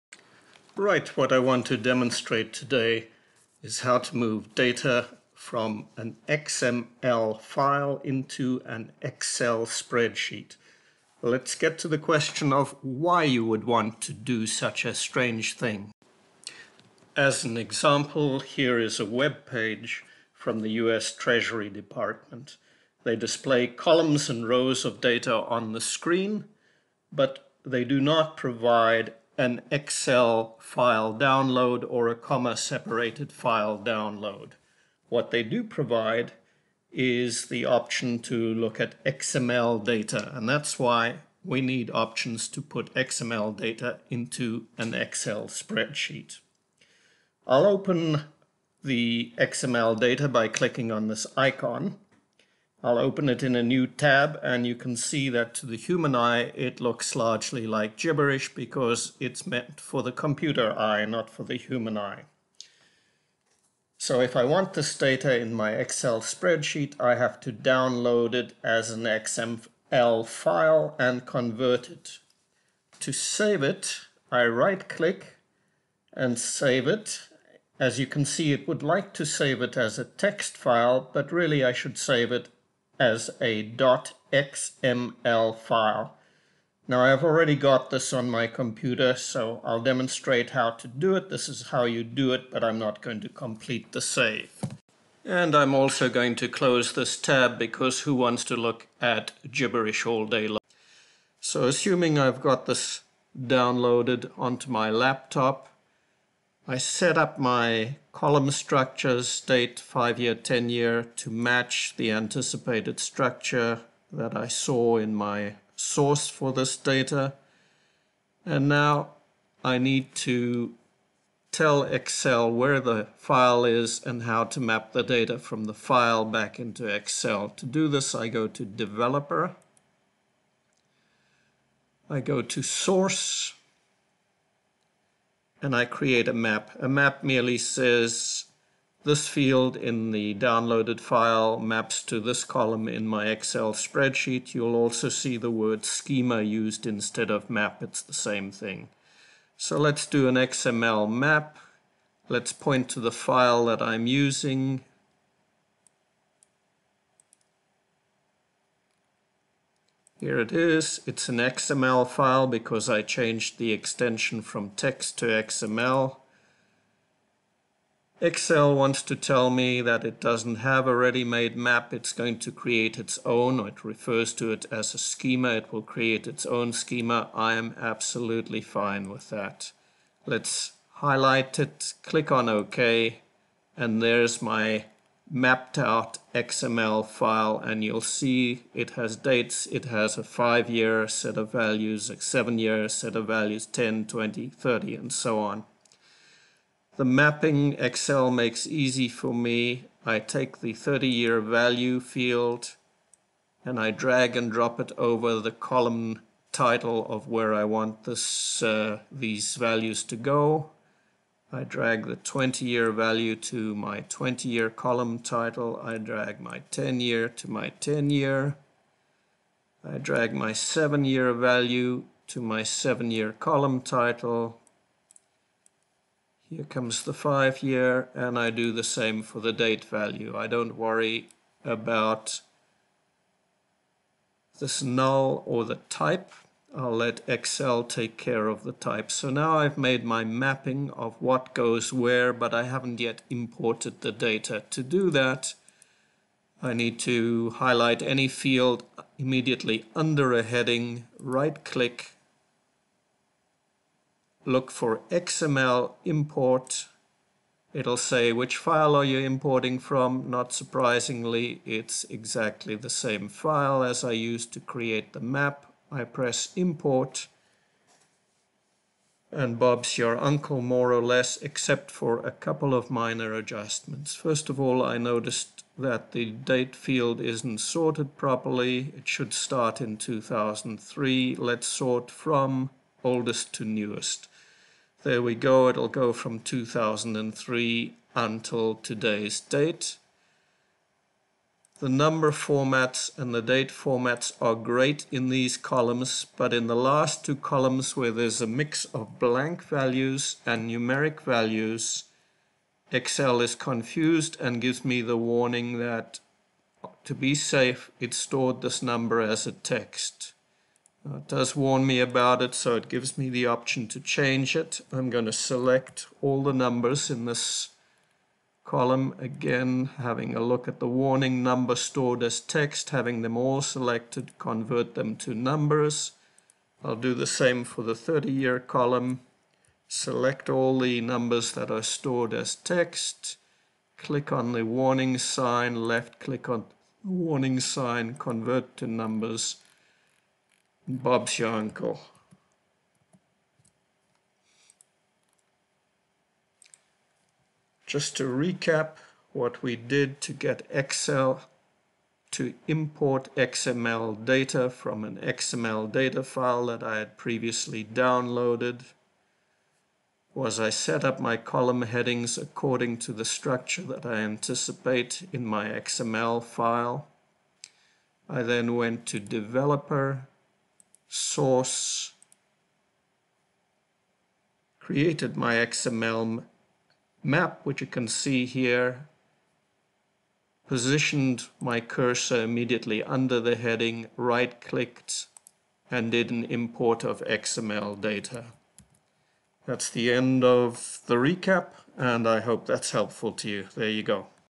Importing XML Data Into Excel
Web pages sometimes provide data as XML files rather than in a convenient format for Excel spreadsheets.

This video shows you how to save XML files, how to reveal their structure (the schema), how to map XML data fields to Excel column headings and how to import the mapped data into Excel.

I hope it's helpful.